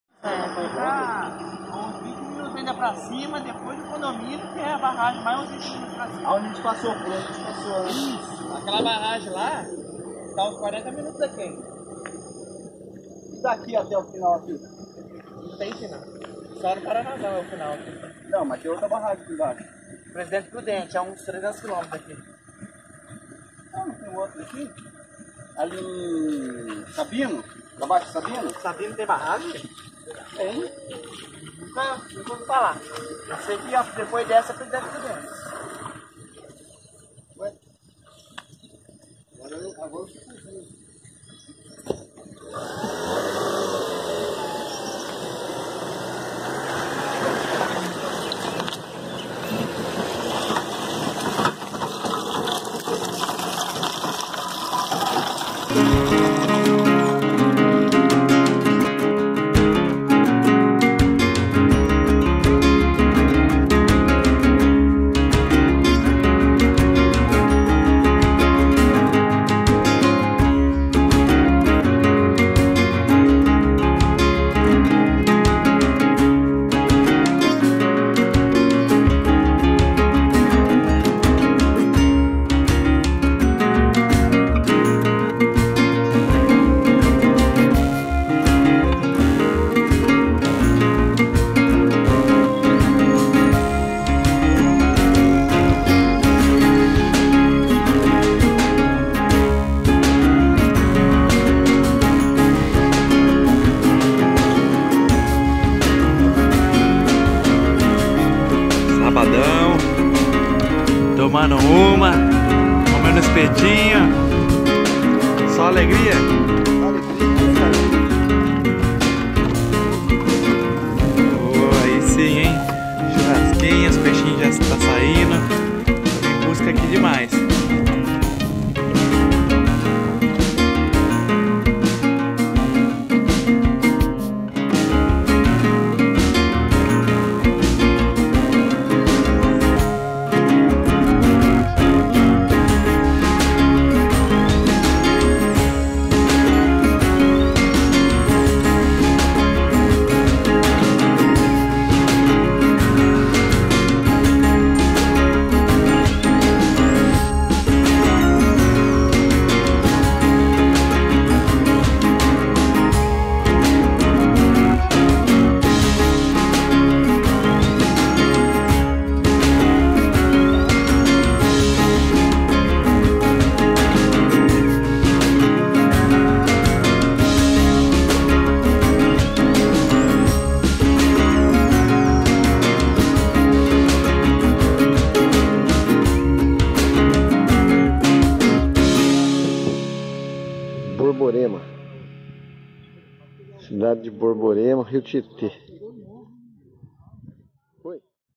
Pescaria em Borborema S. P. Com o piloteiro Jhonatan de Francisco.
Pescaria de corvinas no rio Tietê, Borborema SP.